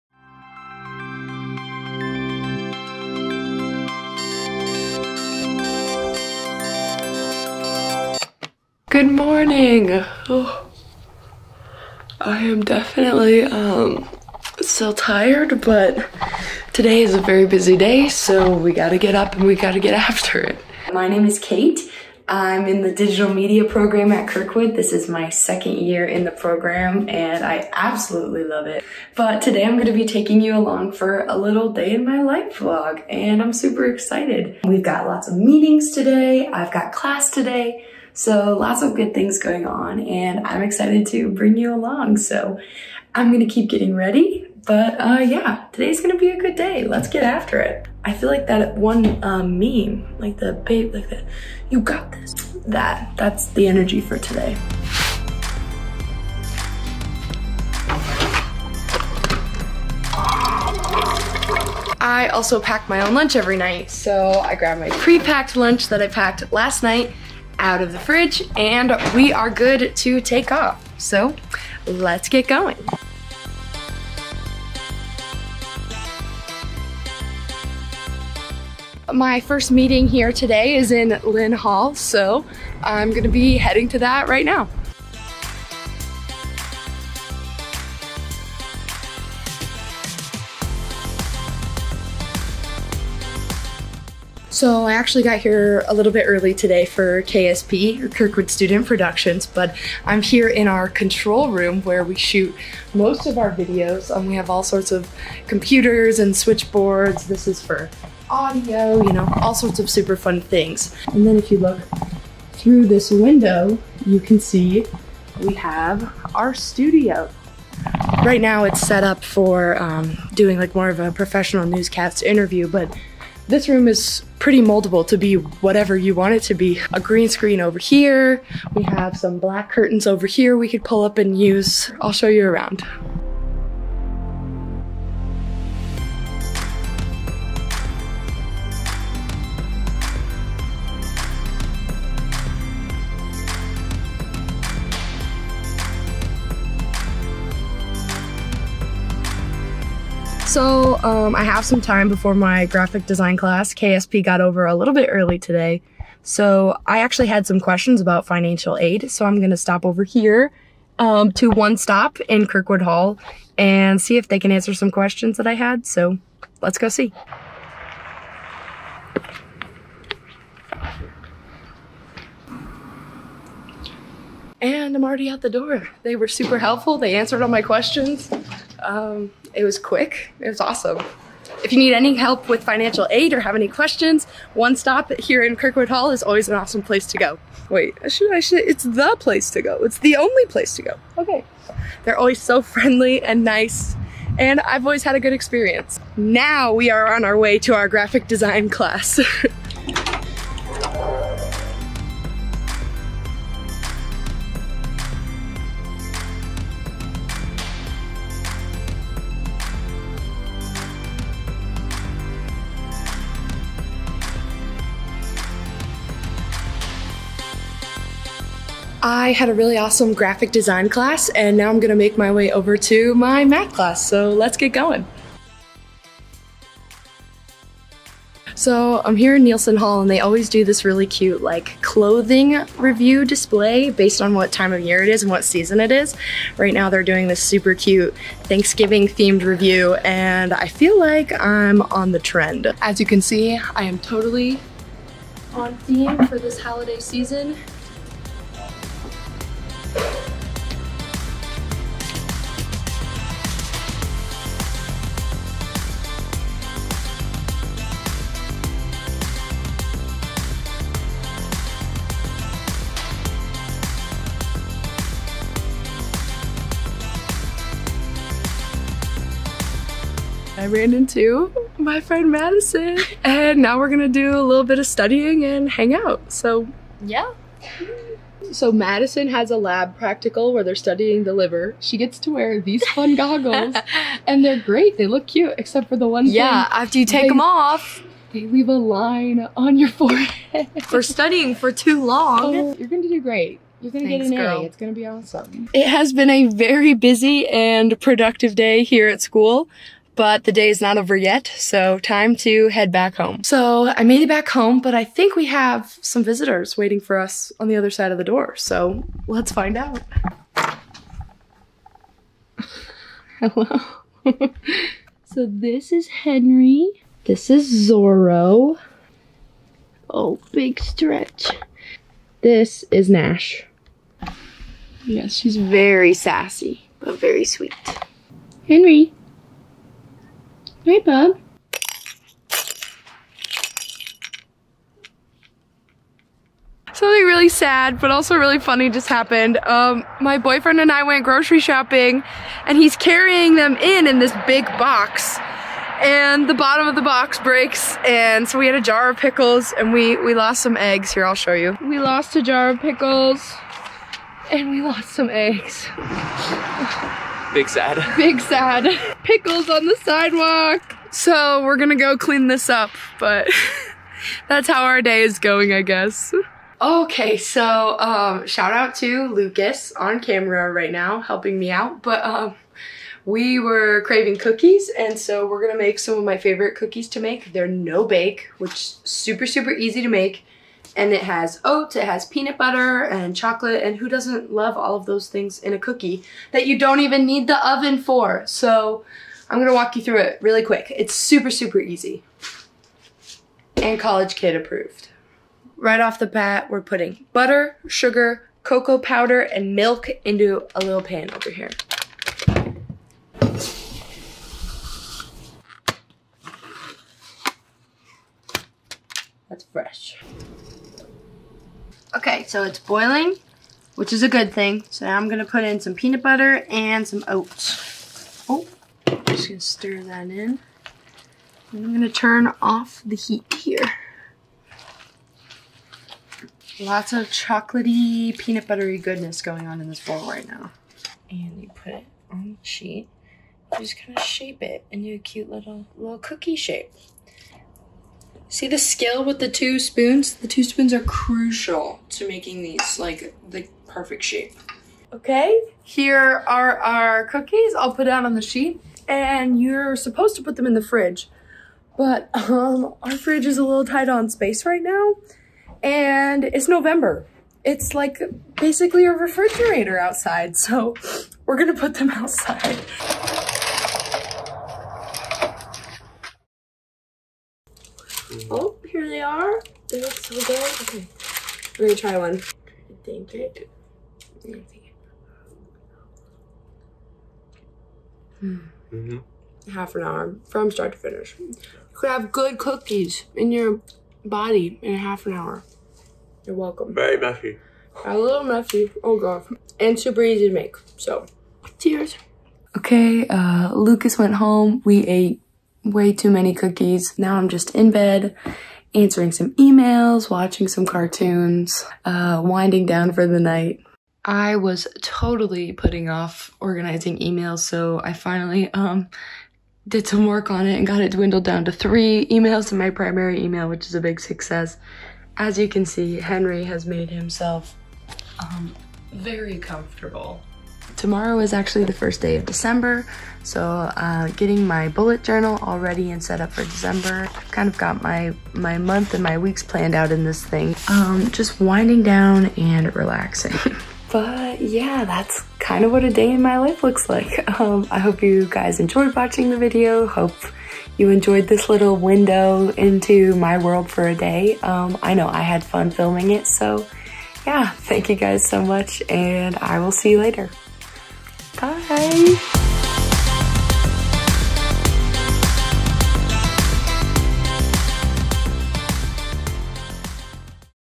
Kirkwood VLOG | A Day in the Life of Kate
Follow Kate – a Digital Media major in her second year at Kirkwood – and get a feel for what life is really like for a Kirkwood student!